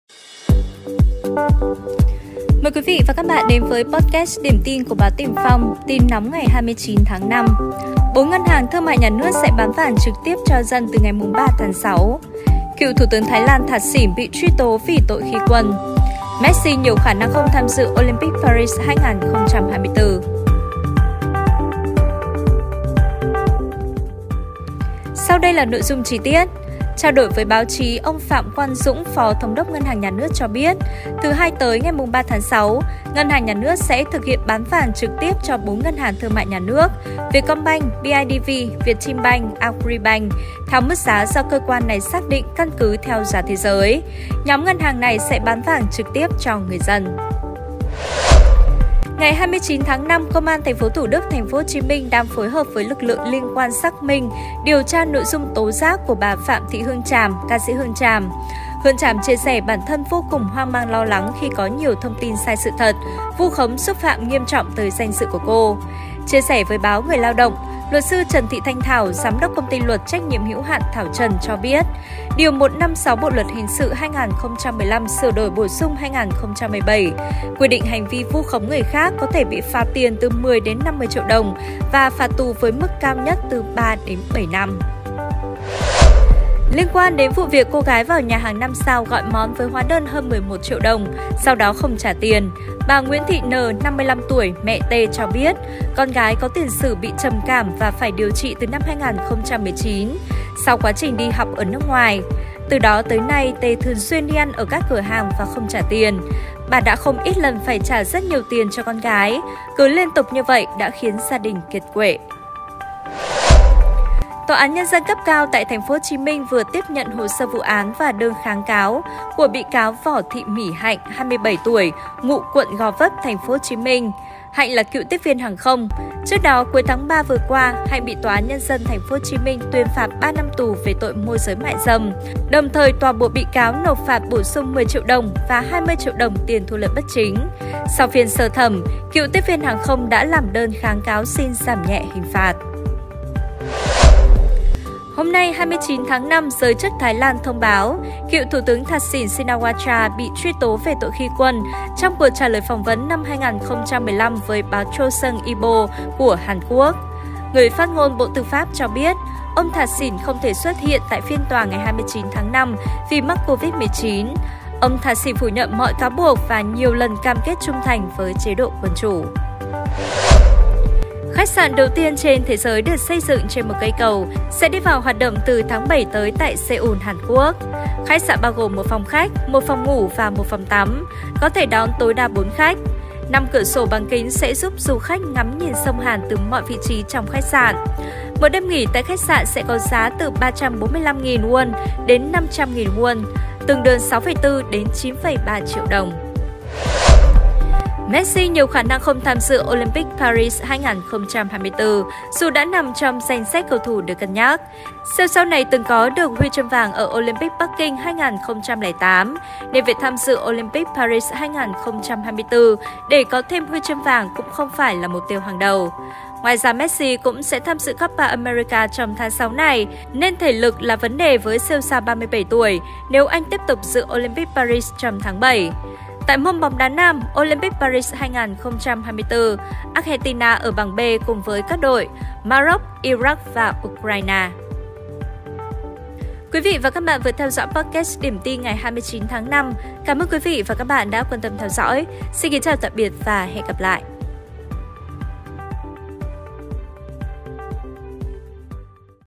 ĐIỂM TIN 29/5: Tin mới vụ cựu tiếp viên hàng không môi giới mại dâm
Tiền Phong TV | TIN NÓNG: 4 ngân hàng thương mại Nhà nước sẽ bán vàng trực tiếp cho dân từ ngày 3/6; Cựu Thủ tướng Thái Lan Thaksin bị truy tố vì tội khi quân; Messi nhiều khả năng không tham dự Olympic Paris 2024.